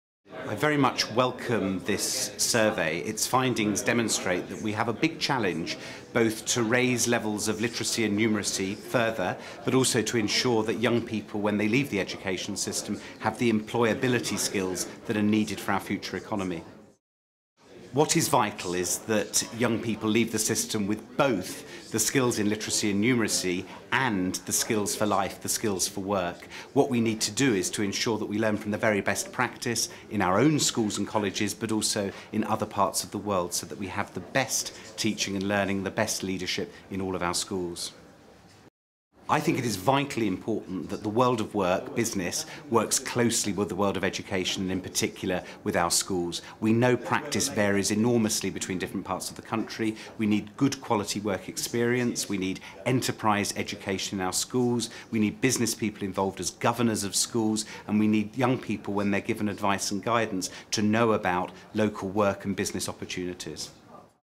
Stephen Twigg MP: It's vital that young people leave school with the skills for work
The shadow education secretary, discusses the findings of the CBI/Pearson Education & Skills Survey, how to improve basic levels of literacy, numeracy and employability skills, and why it's important for businesses and schools to work together.
Find out more about the CBI's work on education and skills